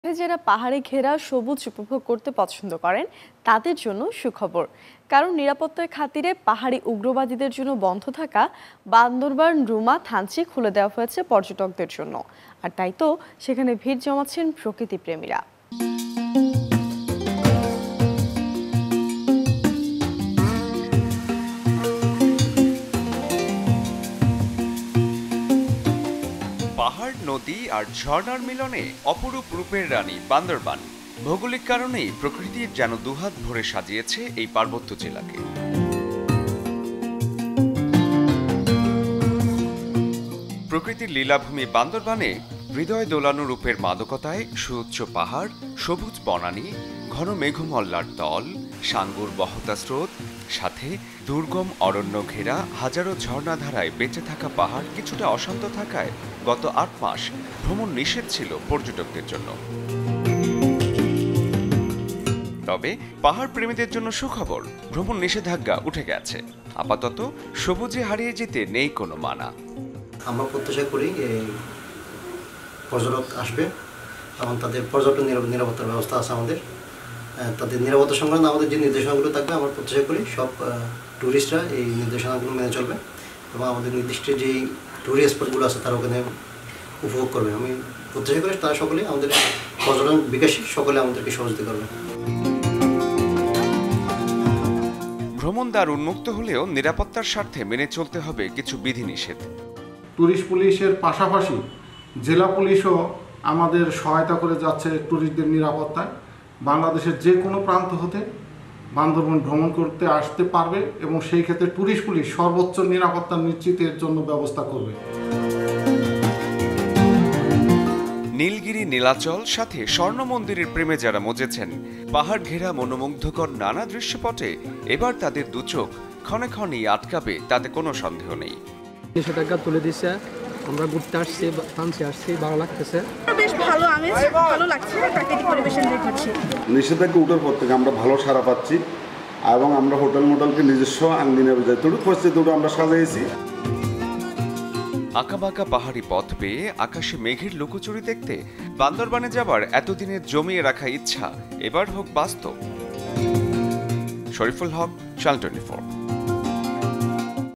আবারও খুলে গেলো বান্দরবানের দরজা, কবে যাচ্ছেন ঘুরতে? | Bandarban | Channel 24
প্রকৃতির লীলাভূমি বান্দরবানে হৃদয় দোলানো রূপের মাদকতায় সুউচ্চ পাহাড়, সবুজ বনানী, ঘন মেঘমল্লার দল, সাঙ্গুর বহতা স্রোত সাথে দুর্গম অরণ্য ঘেরা হাজারো ঝরনা ধারায় বেঁচে থাকা পাহাড় কিছুটা অশান্ত থাকায় গত ৮ মাস ভ্রমণ নিষেধ ছিল পর্যটকদের জন্য। তবে পাহাড়প্রেমীদের জন্য সুখবর, ভ্রমন নিষেধাজ্ঞা উঠে গেছে।

»» Office Address:
Bangladesh.